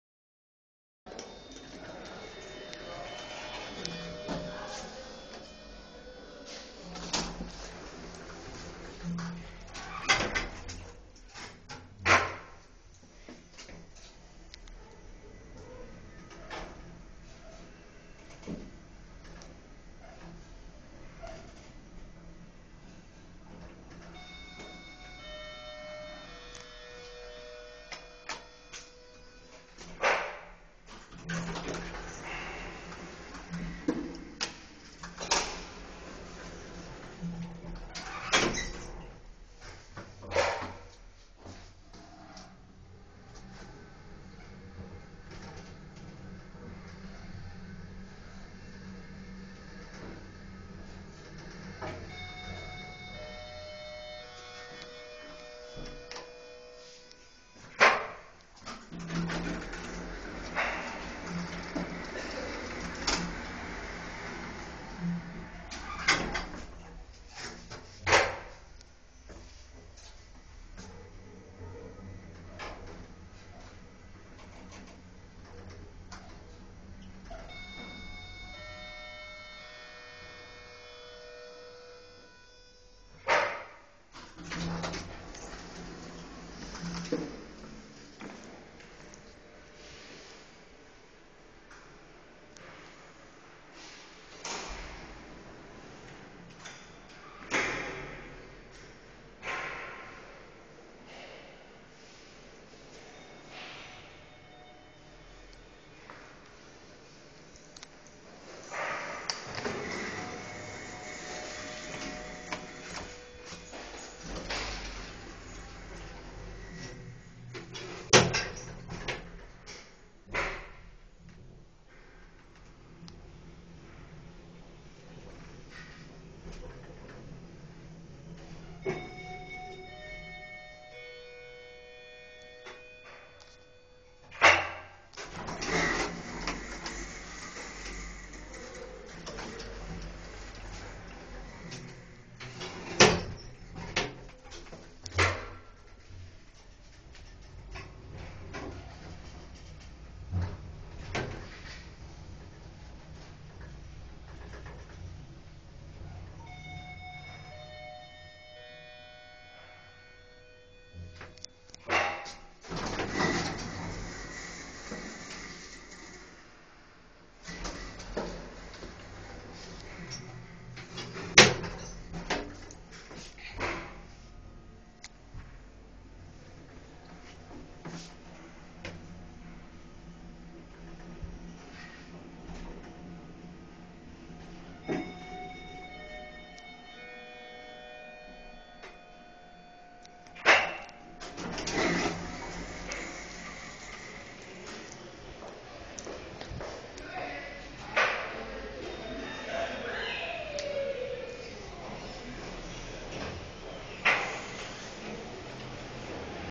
Very nice Elitec Traction (MRL) elevators @ Antonin Poncet Parking Garage, Lyon, France
2019 EDIT : these elevators have been modernized with Schaefer buttons.
I really like these elevators. They run smoothly. They are underground, and do not serve the ground floor. A separate elevator has been built in 2021 to link the ground floor with these main elevators.
Technical information
* Brand : Elitec
* Model : Dewhurst fixtures
* Type : Traction MRL ( machine room less ).
* Capacity : 630 KG / 8 Persons
* Installed in the 2000s
* Floors served: 5
* Bank of 3 elevators